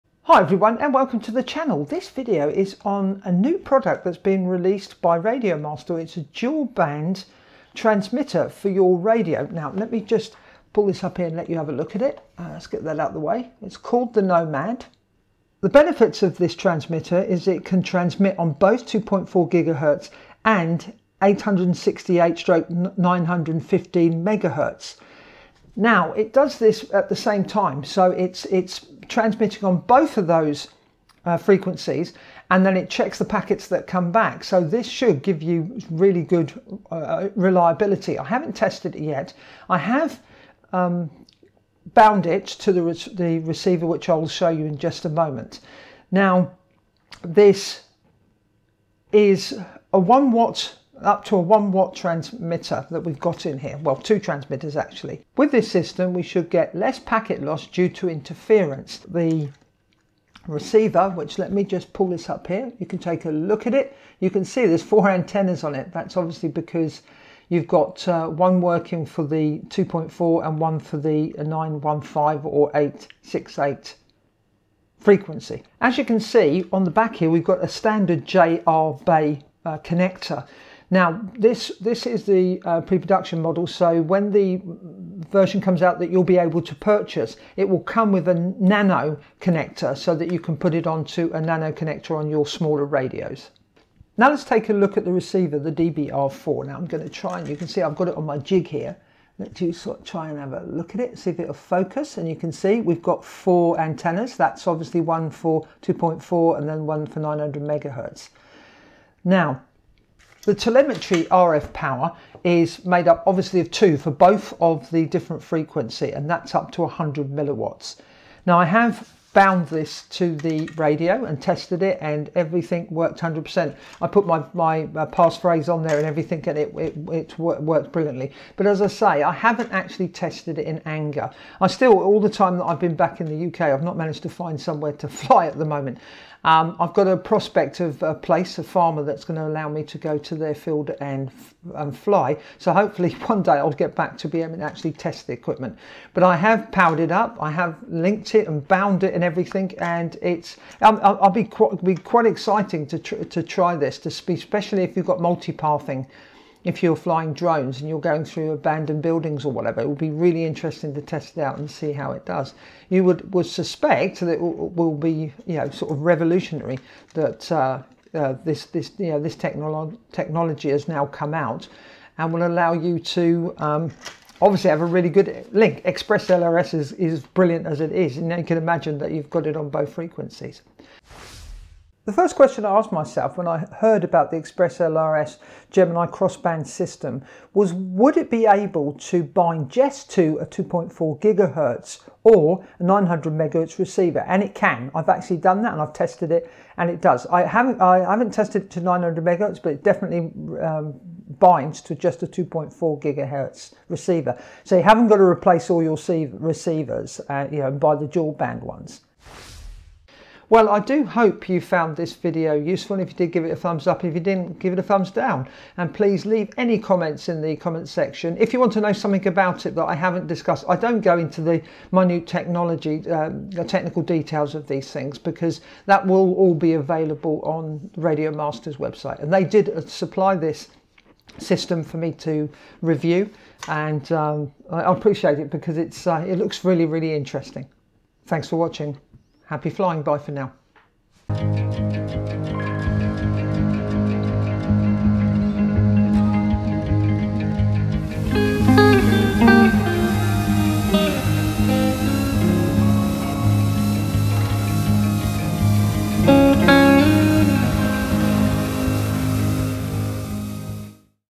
Product Overview RadioMaster Nomad Gemini Xrossband ExpressLRS Module & DBR4 Receiver
This is my overview of the RadioMaster Nomad Gemini Xrossband ExpressLRS Module and DBR receiver. This is an exciting development of Express LRS technology with 2 frequencies in one transmitter which should give a very reliable link between the transmitter and receiver.

Please visit RadioMasters website for more information.

Happy Flying.